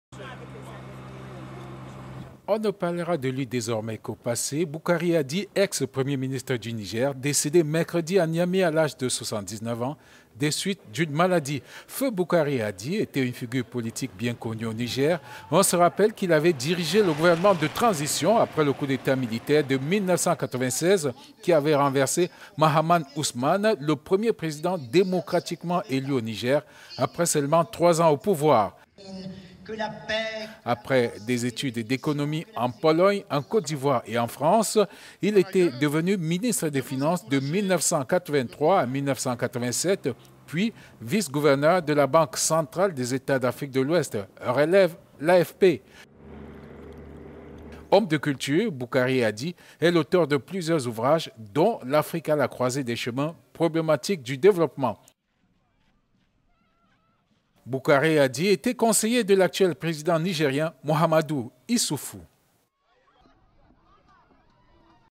Niger : mort de l'ancien Premier ministre Boukary Adji
On ne parlera de lui désormais qu'au passé, Boukary Adji, ex-Premier ministre du Niger, décédé mercredi à Niamey à l'âge de 79 ans  des suites d'une maladie.

Figure politique très connue, il avait dirigé le gouvernement de transition après le coup d'Etat militaire de 1996 qui avait renversé Mahamane Ousmane, le premier président démocratiquement élu du Niger, après trois ans au pouvoir.

Après des études d’économie en Pologne, en Côte d’Ivoire et en France, il était devenu ministre des Finance…